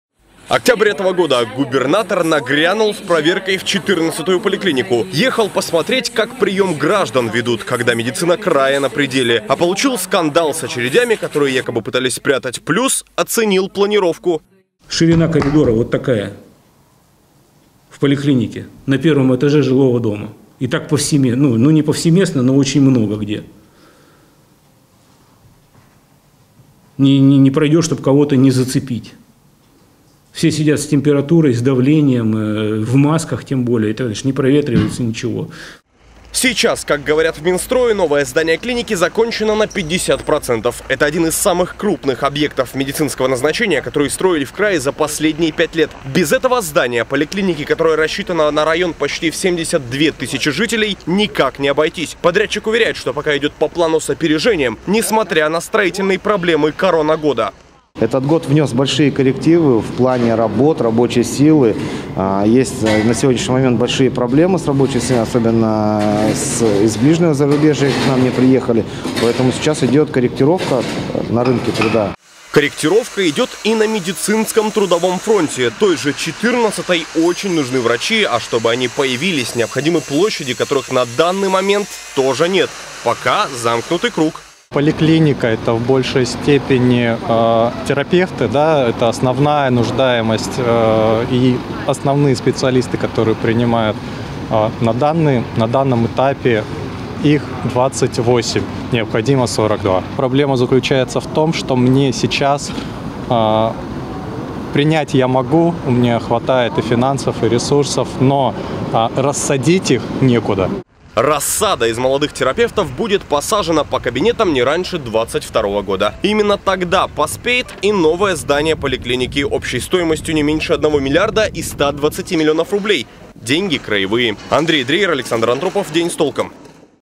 Готово на половину: когда в Барнауле откроют новый корпус поликлиники №14
70 тысяч барнаульцев ждут не дождутся, когда завершится строительство нового корпуса  14-й  поликлиники. Напомним, в октябре в период резкого подъема количества больных коронавирусом, именно в этом учреждении  побывал с проверкой губернатор. По его словам, 14-я «потрясла»: несколько разбросанных  корпусов, узкие коридоры, духота.  Когда ситуация изменится и в новом здании начнут принимать пациентов?